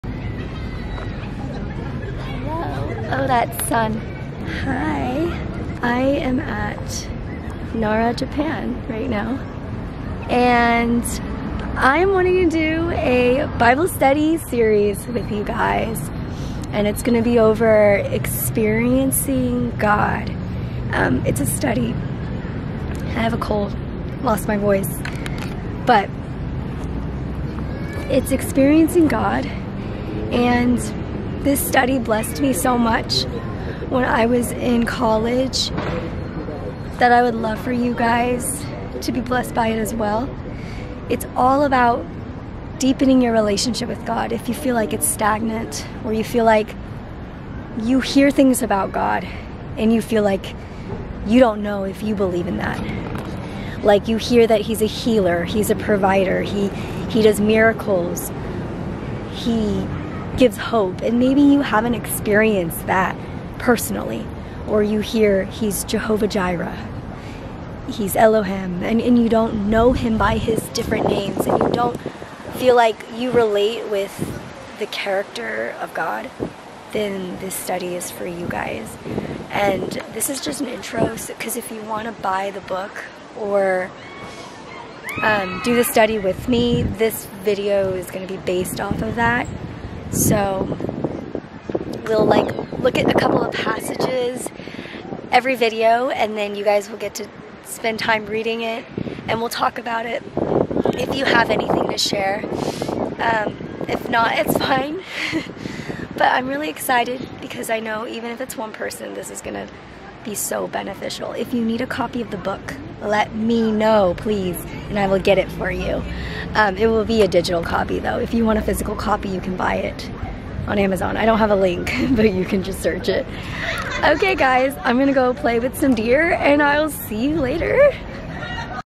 Wanna Bible Study Together?
The video "study" will be based on Experiencing God - they have a book and a bible study/devotional book. You can still watch the videos even if you don't own the book or do not want to read it.

How long will this bible study be?
I'm honestly figuring it out as I go! The book has 26 chapters so that's unrealistic to make 26 sessions. The work book varies from 12 to 8 weeks. It really depends what version you have and what year edition (I currently have a youth version I got in college around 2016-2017). So the answer is... Imma let the Lord guide me as I film the videos. Even figuring out what to include is a challenge I don't want to think about too hard.

How do I prepare?
You can read the devotional everyday or you can read a chapter every day from the book - or you can just hop on the video and see what bible passages are included and study those! I will try to post on my Instagram about these passages~  Also, make sure your heart is in the right place and you are in prayer as you study the bible. This bible study video series should not replace real human interaction and being involved in a community~ It is merely supplemental. :)